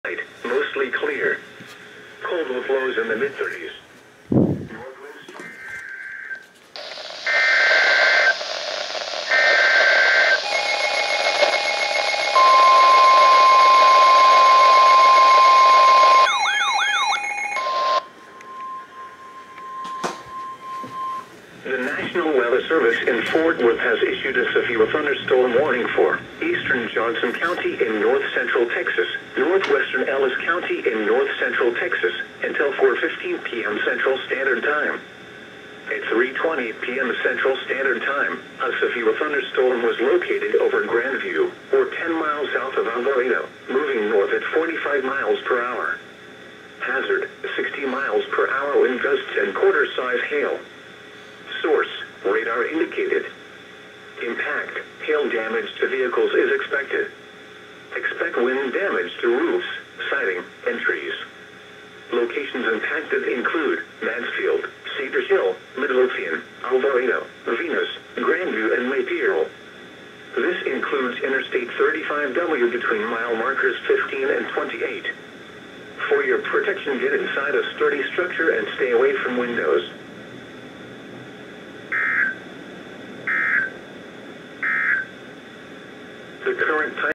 Severe Thunderstorm Warning (EAS #1,914-1,915) - December 26, 2018
Severe Thunderstorm Warning for Ellis and Johnson counties in Texas. - KEC55, KEC56

===LINKS===